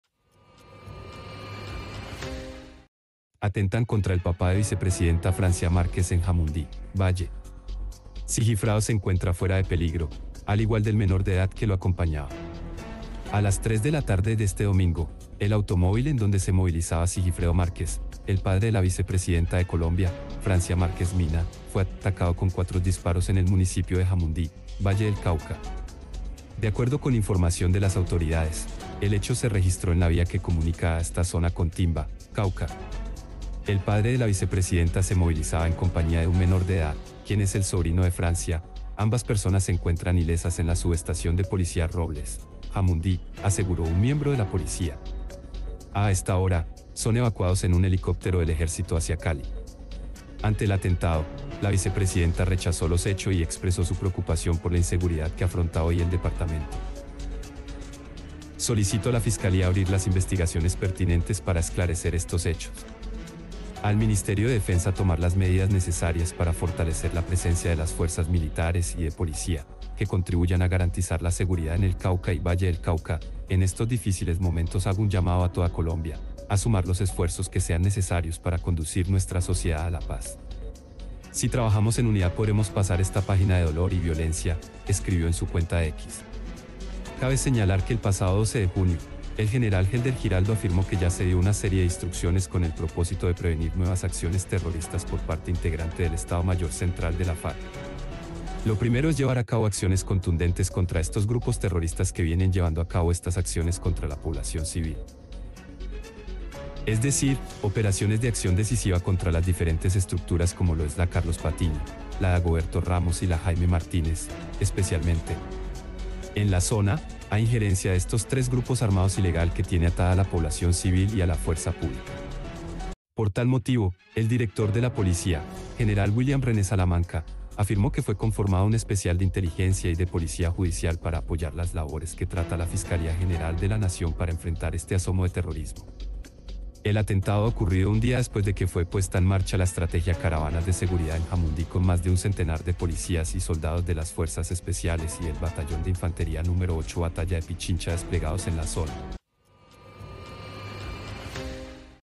ÚLTIMA HORA 🔴 Atentan contra el papá de vicepresidenta Francia Márquez en Jamundí, Valle
De acuerdo con información de las autoridades, el hecho se registró en la vía que comunica a esta zona con Timba (Cauca). “El padre de la vicepresidenta se movilizaba en compañía de un menor de edad (quien es el sobrino de Francia), ambas personas se encuentran ilesas en la subestación de Policía Robles - Jamundí”, aseguró un miembro de la Policía. A esta hora, son evacuados en un helicóptero del Ejército hacia Cali.